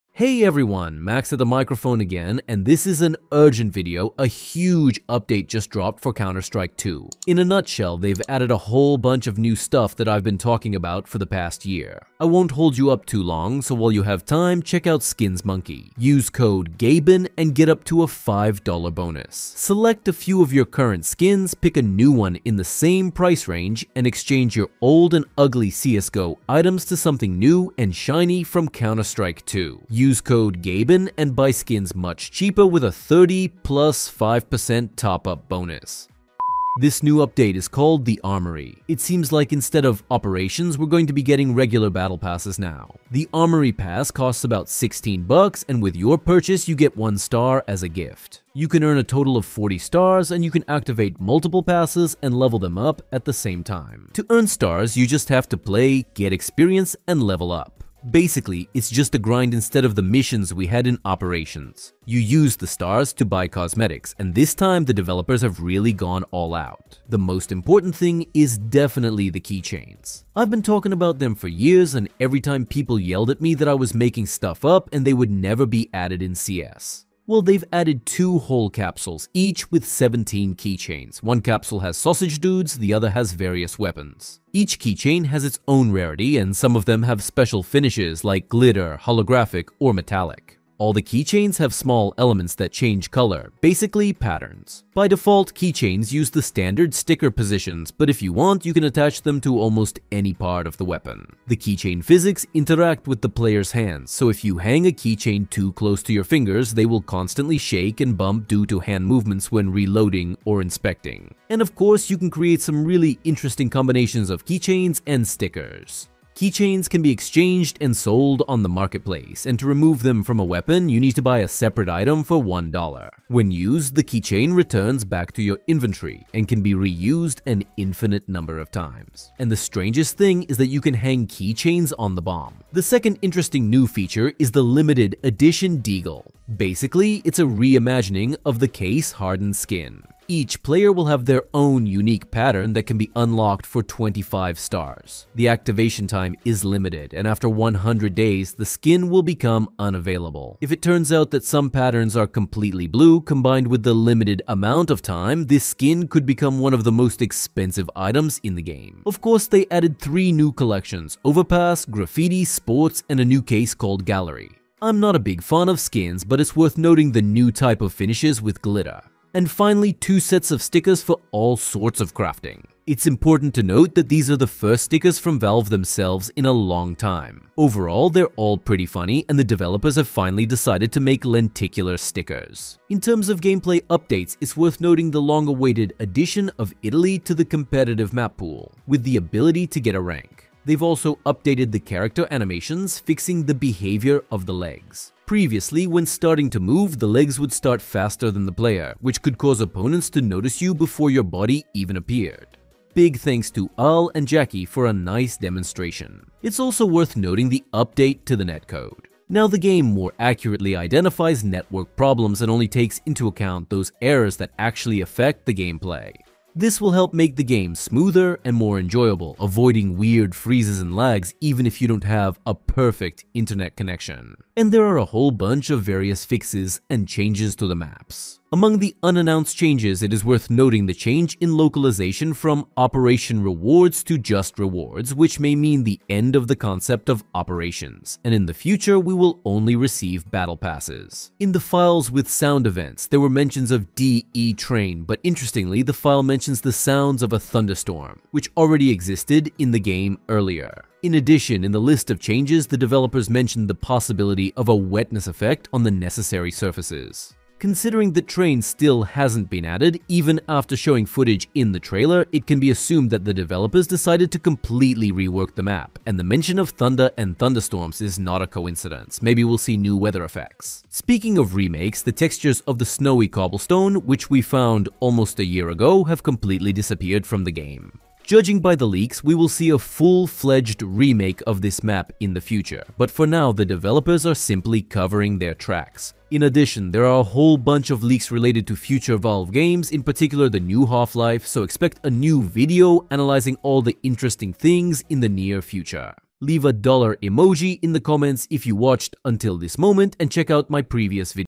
New CS2 Update - Keychains / Battle Pass / Train Remake / Operation?
CS2 and CS:GO items / CODE: GABEN

In this video I'll talk about a new CS 2 update with Battle Pass similar to previous CS:GO operation / Keychains or Charms / Leak of the Train remake